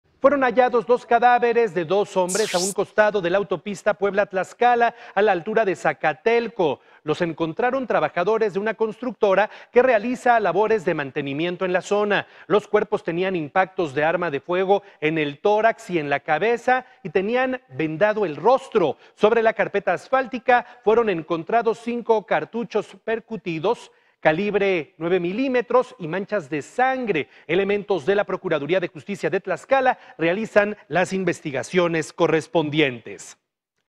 Hallan cuerpos || Noticias con Juan Carlos Valerio
Fueron hallados los cuerpos de dos hombres a un costado de la autopista Puebla-Tlaxcala, a la altura de Zacatelco.